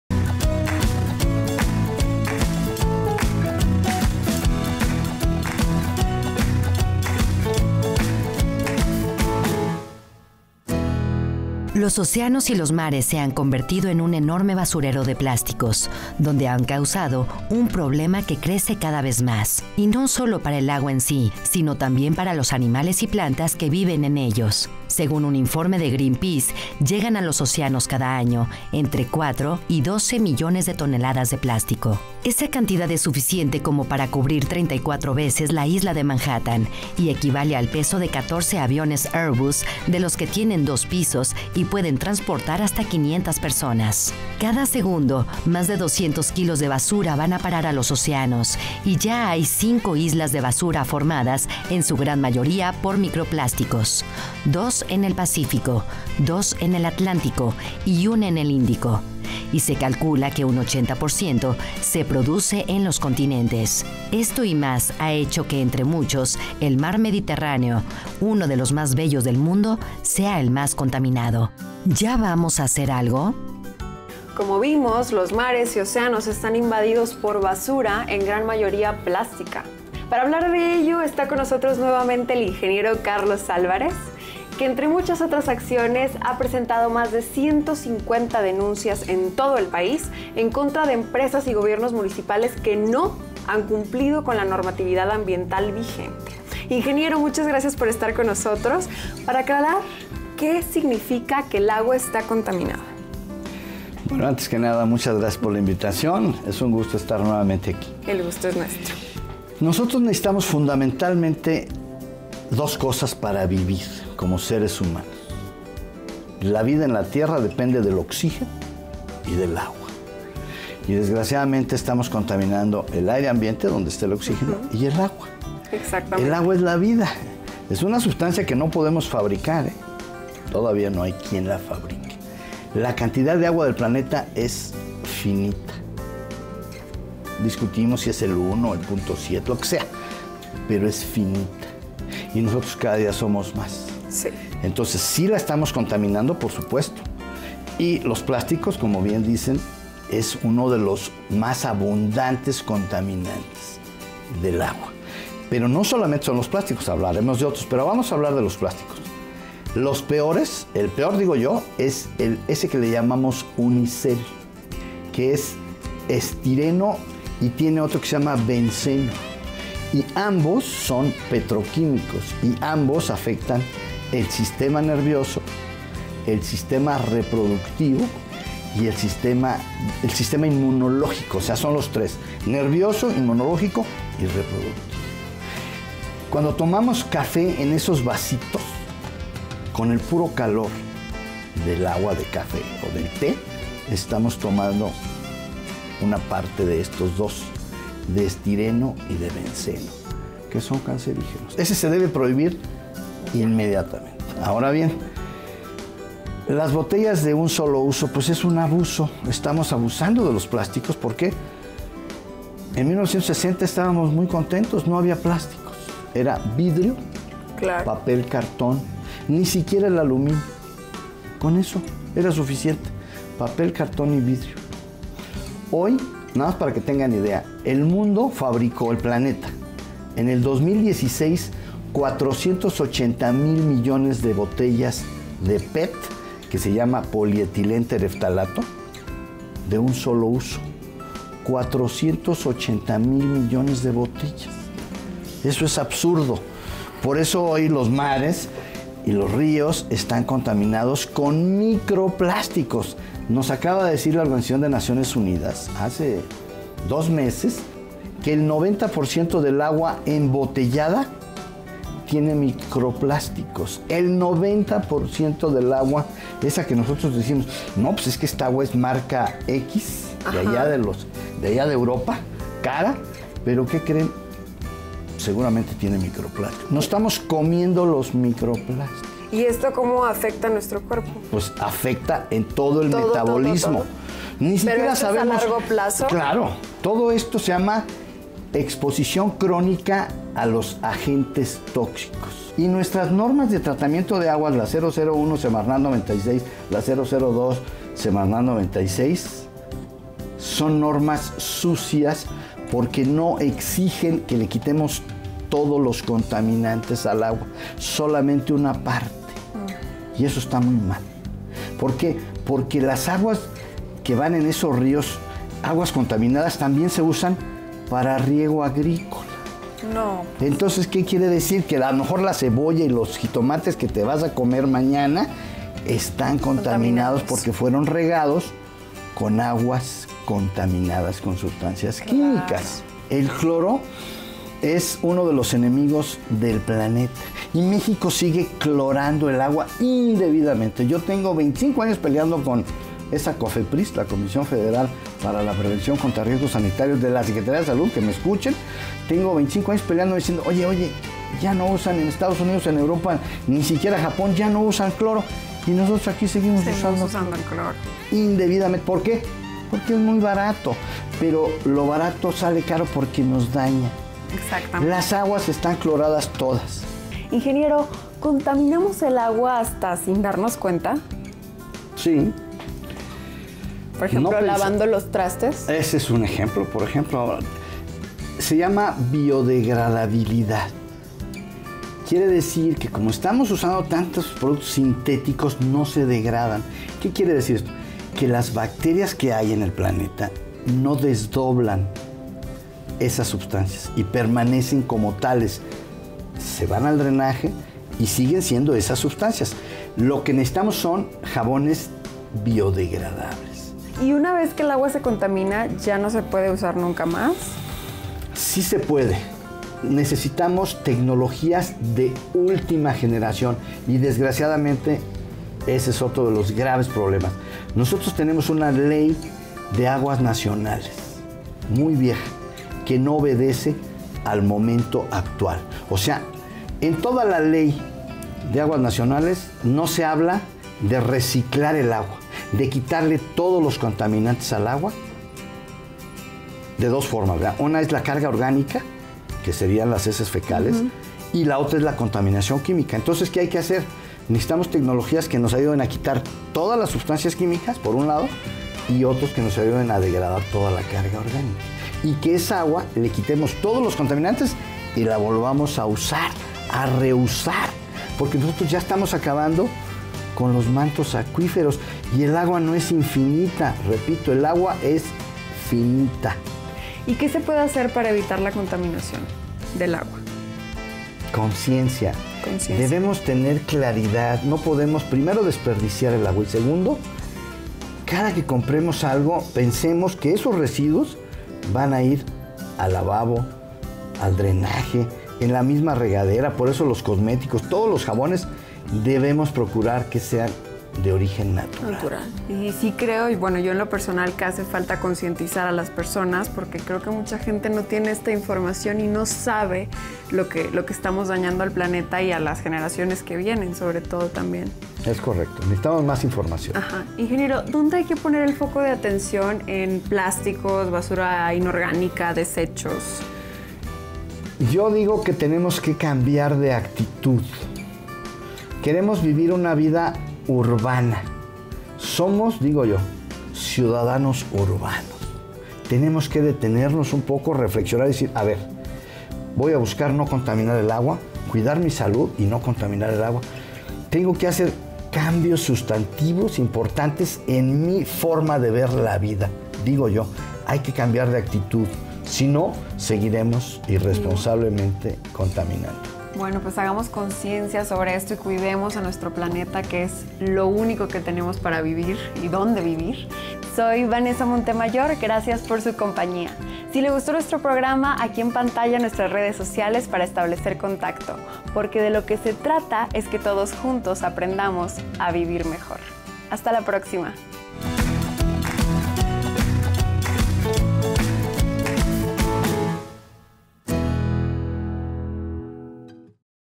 ¡Cuidemos el agua!
El ingeniero Carlos Álvarez nos dice los riesgos de la contaminación del agua.